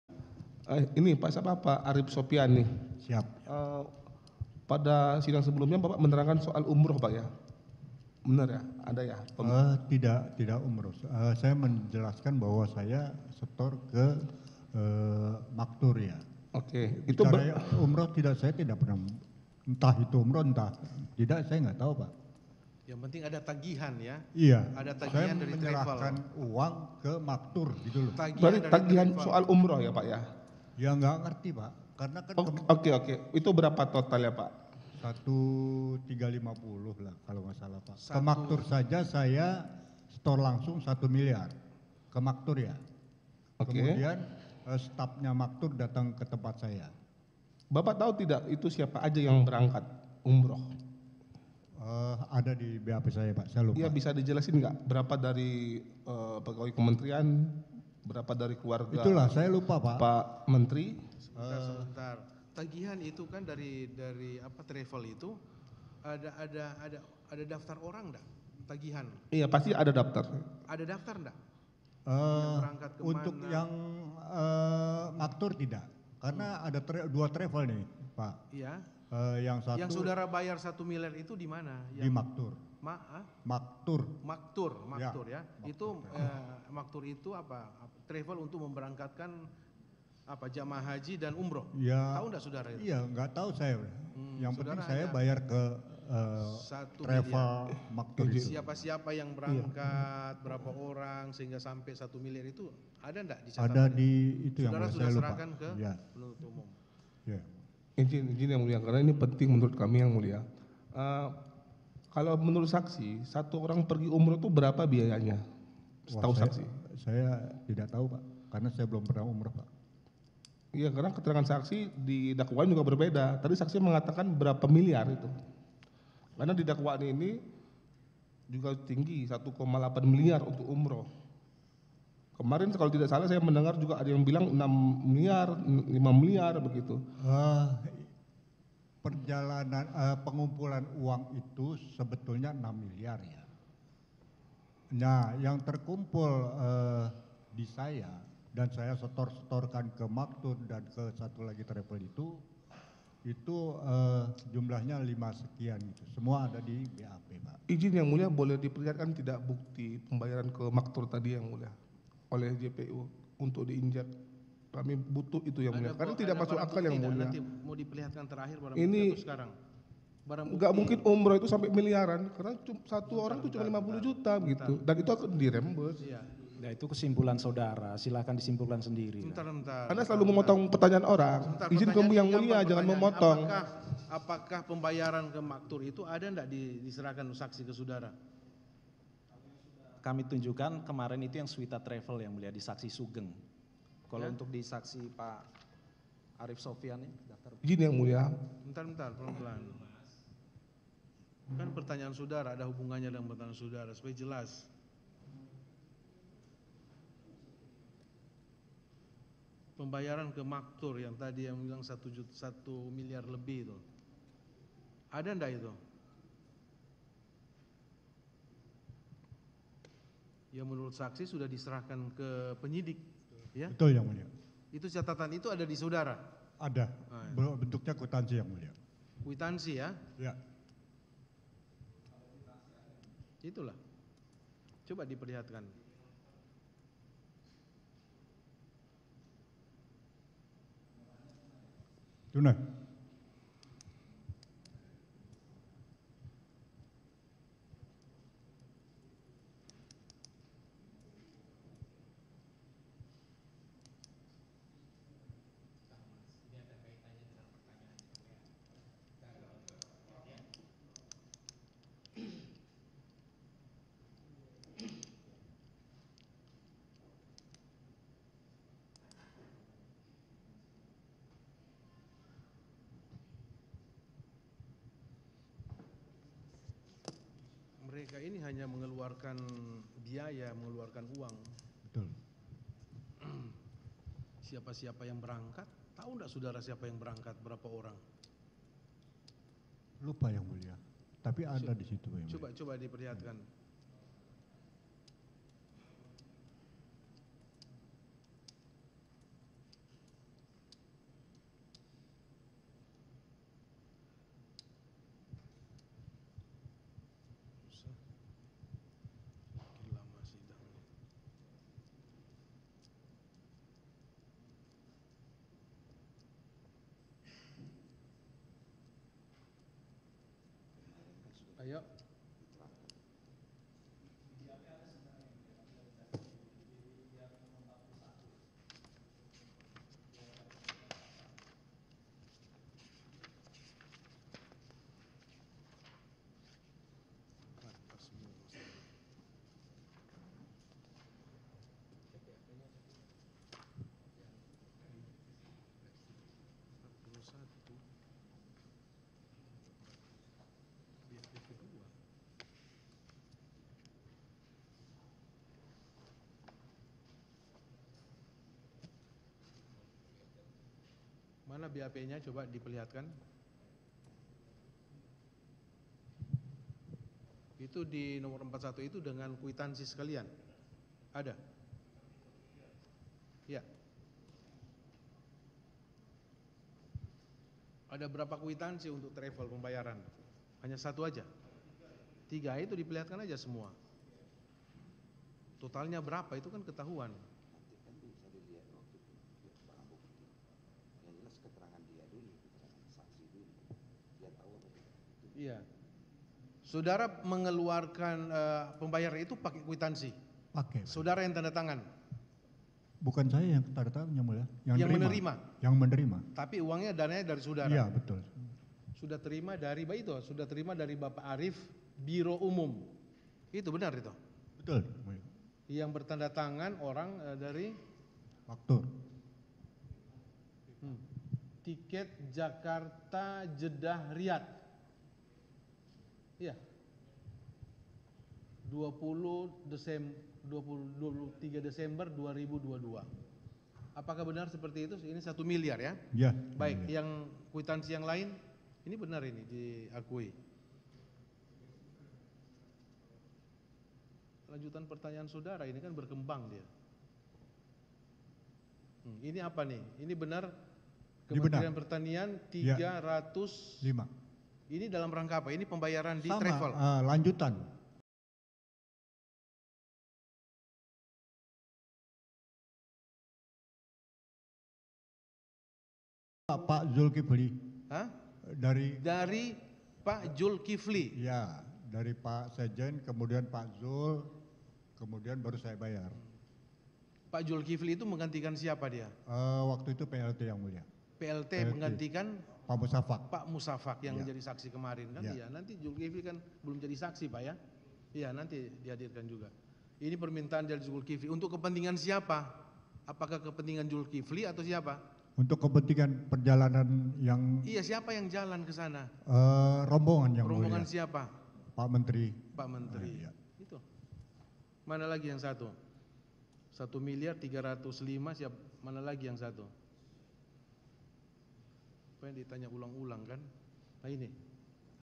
Panas! Debat Kuasa Hukum dan Jaksa soal Dana Umrah SYL Capai Miliaran di Sidang
JAKARTA, KOMPAS.TV - Kuasa hukum mantan Menteri Pertanian Syahrul Yasin Limpo (SYL) terlibat perdebatan dengan jaksa penuntut umum soal dana umrah yang dipakai terdakwa SYL di Sidang lanjutan kasus korupsi, PN Tipikor, Jakarta, (6/5/2024).

Awalnya, kuasa hukum SYL bertanya ke saksi terkait keterangan yang menyebut biaya umrah SYL mencapai miliaran rupiah.

"Nggak mungkin umroh itu sampai miliaran, 1 orang itu cuma 50 juta dan itu di reimburse," ujar kuasa hukum SYL.

"Itu kesimpulan saudara, silakan simpulkan sendiri," balas jaksa penuntut umum.

"Anda selalu memotong pertanyaan orang," ujar kuasa hukum SYL.

Produser: Ikbal Maulana
Thumbnail: Vila Randita